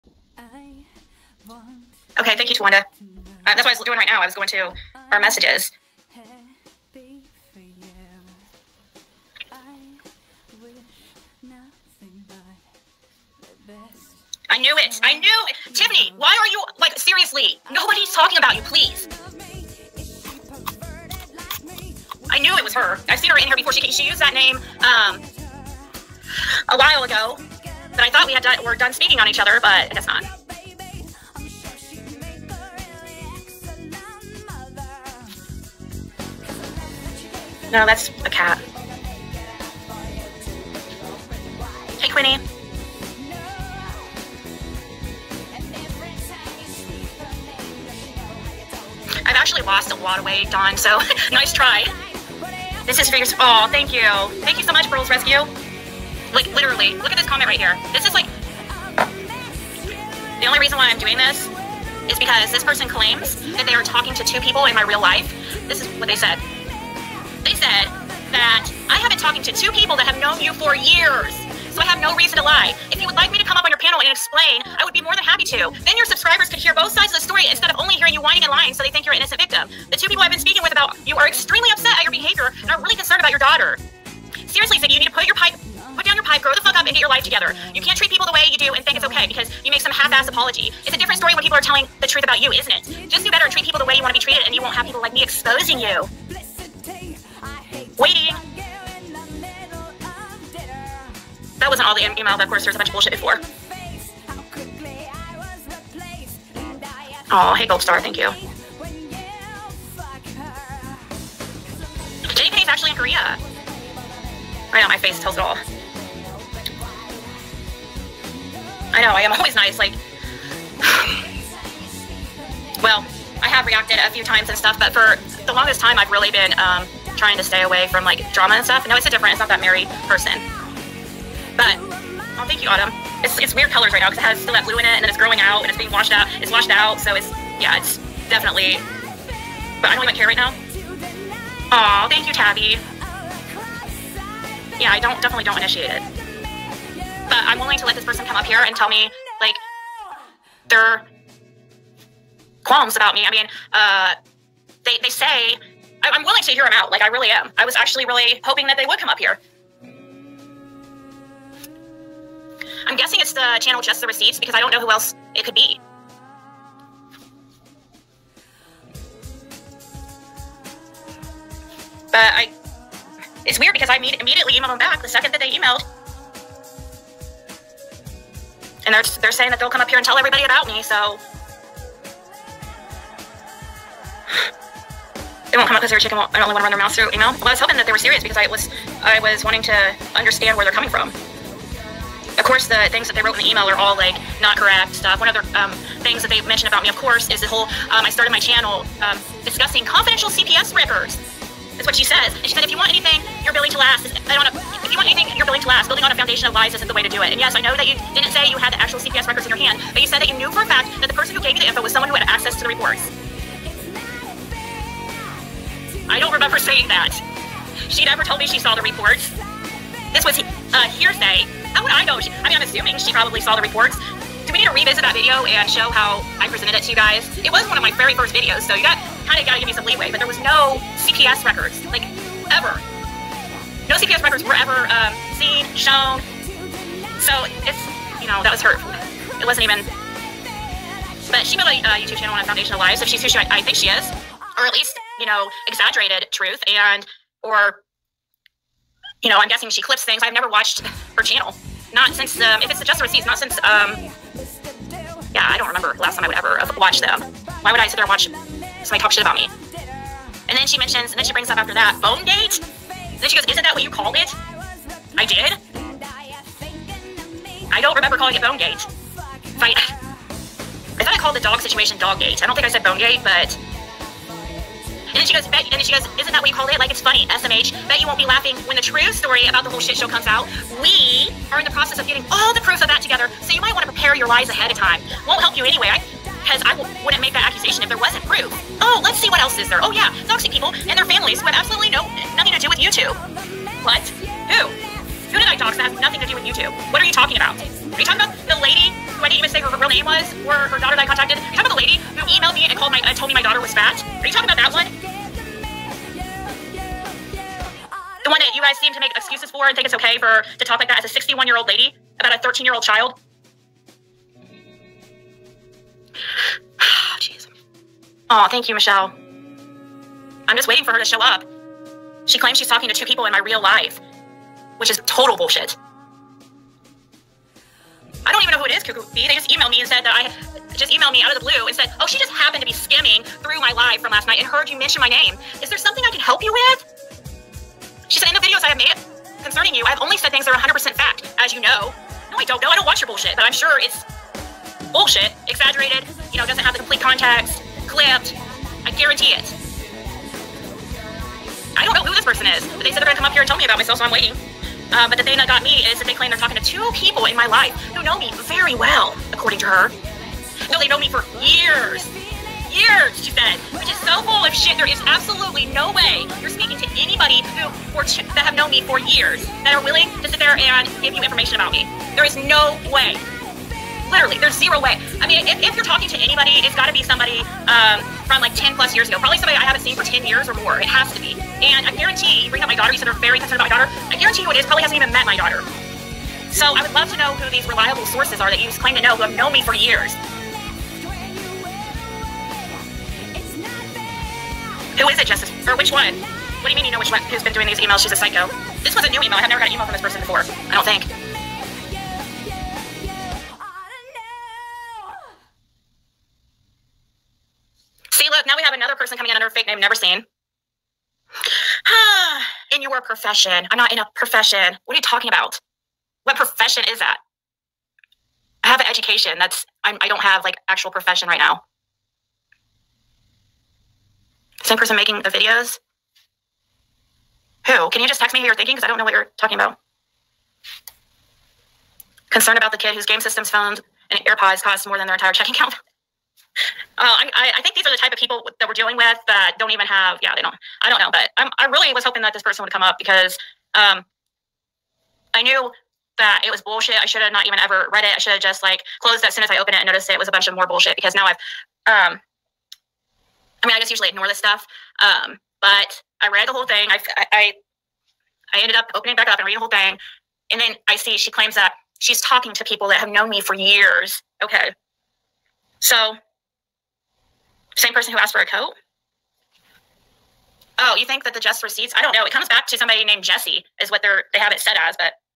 Ziggy Gets Threats to Expose Her For The Liar She Is
Ziggy has allegedly received emails from someone who wants to interview people from her past